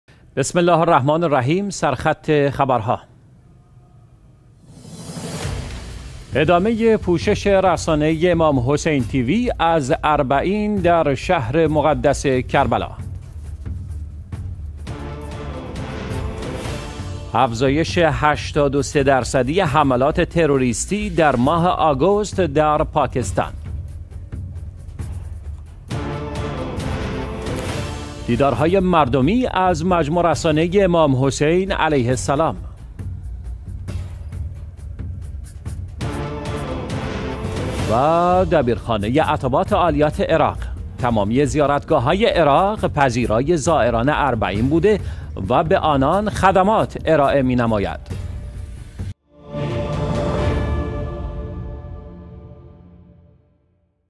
سرخط خبرها: دوشنبه 13 شهریور ماه 1402
۱- ادامه پوشش رسانه ای امام حسین TV از اربعین در شهر مقدس کربلا
۲- افزایش 83 درصدی حملات تروریستی در ماه آگوست در پاکستان
۳- دیدارهای مردمی از مجموعه رسانه ای امام حسین علیه السلام
۴- دبیرخانه عتبات عالیات عراق: تمامی زیارتگاه های عراق پذیرای زائران اربعین بوده و به آنان خدمات ارائه می نماید
۵- آیت الله العظمی شیرازی: پیامبر اکرم صلی الله علیه و آله در ابتدای بعثت بر تساهل و تسامح بعنوان بنیان دین، و امام سجاد علیه السلام در مجلس یزید، بر سَماحت بعنوان یکی از ویژگی های معصومین تاکید فرمودند